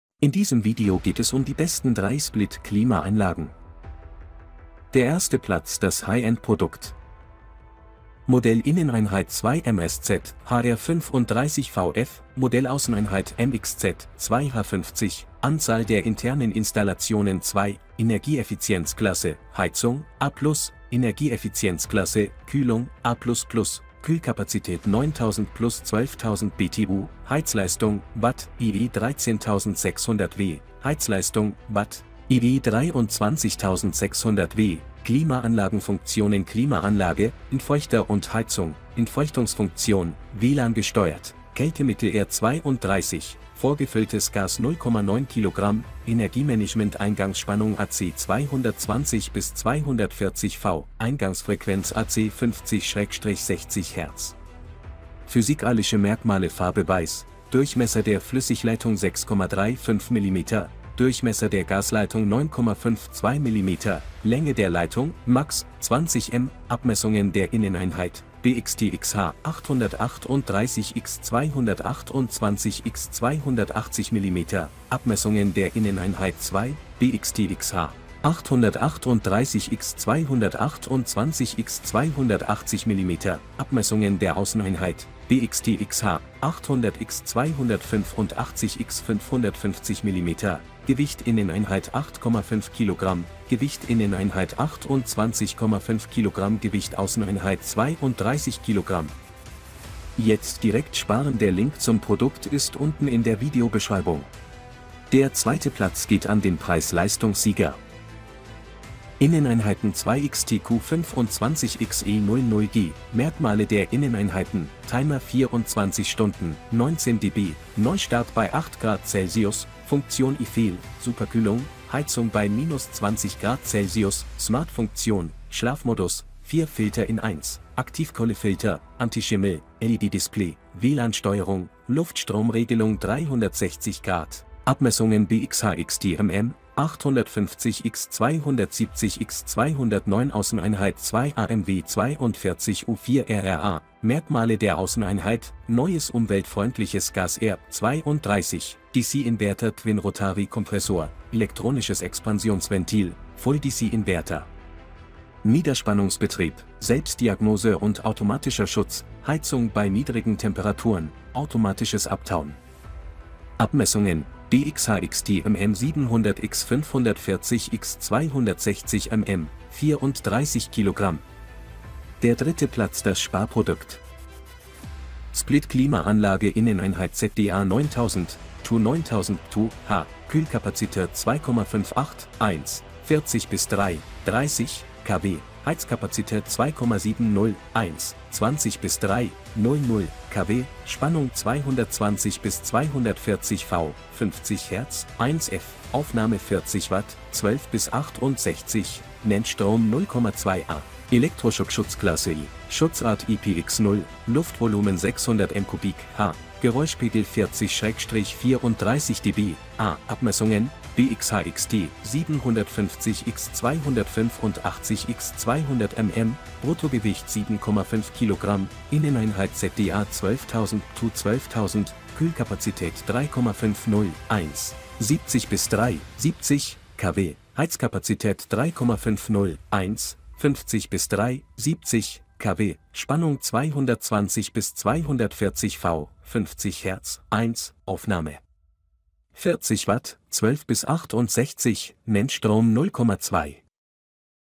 Split Klimaanlage Test - Auf was achten bei dem Kauf?
🔥TOP 3 Split Klimaanlage Test – Überprüfe jetzte den Preis:‍🔥

In diesem Video geht es um die besten 3 Split Klimaanlagen.

Der erste Platz das High End Produkt.

Modell Inneneinheit 2MSZ-HR35VF; Modell Außeneinheit MXZ-2HA50; Anzahl der internen Installationen 2; Energieeffizienzklasse (Heizung) A+; Energieeffizienzklasse (Kühlung) A++; Kühlkapazität 9000 + 12000 BTU; Heizleistung (Watt) IE 13600 W; Heizleistung (Watt) IE 23600 W; Klimaanlagenfunktionen Klimaanlage, Entfeuchter und Heizung; Entfeuchtungsfunktion; WLAN-gesteuert; Kältemittel R32; Vorgefülltes Gas 0,9 kg; Energiemanagement | Eingangsspannung AC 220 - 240 V; Eingangsfrequenz AC 50/60 Hz; Physikalische Merkmale Farbe Weiß; Durchmesser der Flüssigleitung 6,35 mm; Durchmesser der Gasleitung 9,52 mm; Länge der Leitung (max) 20 m; Abmessungen der Inneneinheit (BxTxH) 838 x 228 x 280 mm; Abmessungen der Inneneinheit 2 (BxTxH) 838 x 228 x 280 mm; Abmessungen der Außeneinheit (BxTxH) 800 x 285 x 550 mm; Gewicht Inneneinheit 8,5 kg; Gewicht Inneneinheit 28,5 kg Gewicht Außeneinheit 32 kg.

Der zweite Platz geht an den Preis Leistungs Sieger.

Der dritte Platz das Sparprodukt.